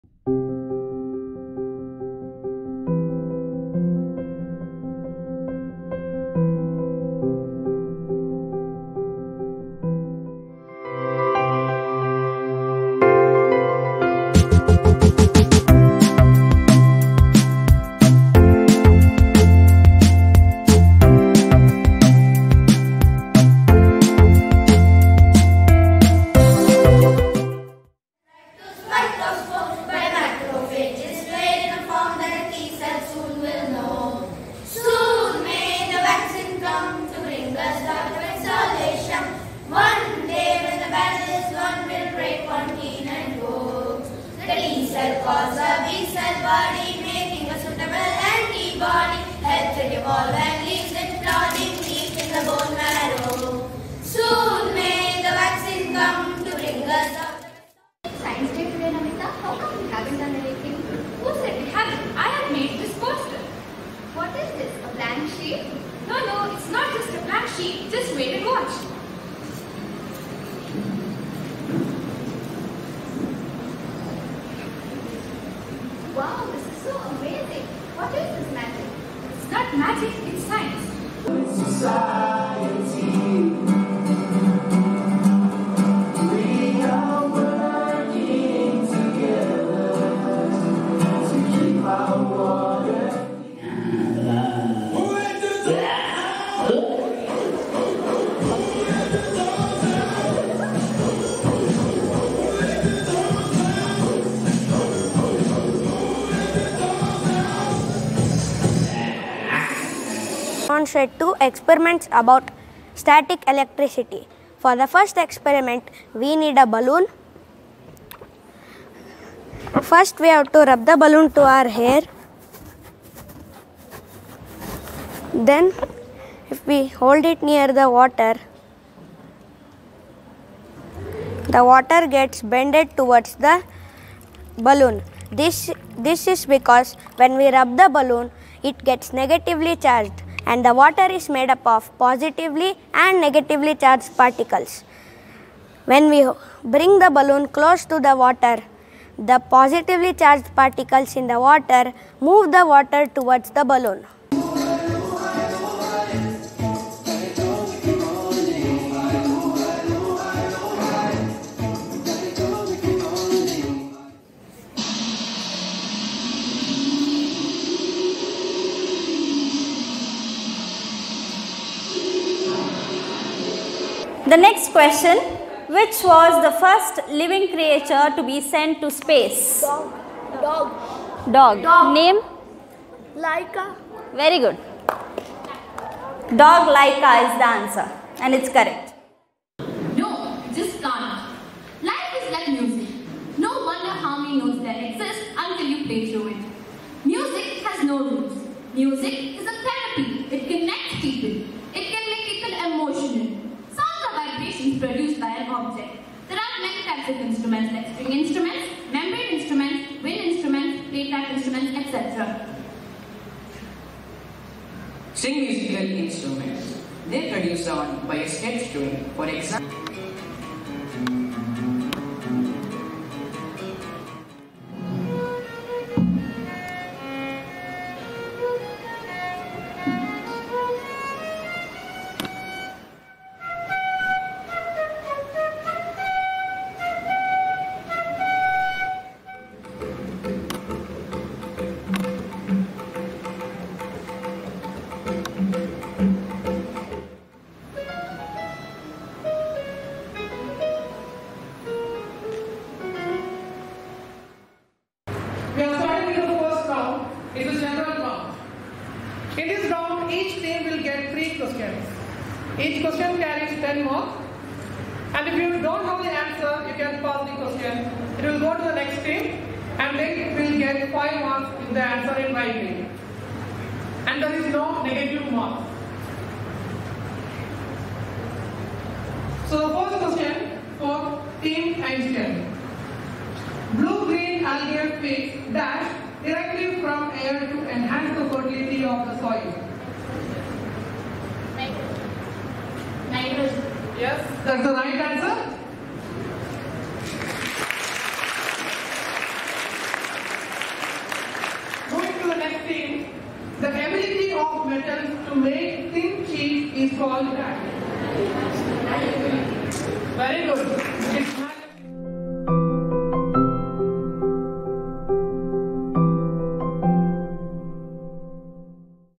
SCIENCE DAY CELEBRATION 2022 at Chitrakoota School, Bangalore
With palpable excitement  resounding in the Auditorium, children waited with bated breath to be a part of SCIENCE DAY CELEBRATION on the 28th of February, 2022. Aided by our Science teachers, right from the 3rd to the 8th graders, put up a fantastic show; some breaking the myths, some demonstrating experiments, Mime and songs were rendered as well. A quiz was conducted to re-kindle the scientific mind of our students and the best team walked away with the prize. Audience clamored to give the right answers. Quite an  honor  to celebrate the birth anniversary of one our greatest scientists of India, Sir C.V.Raman.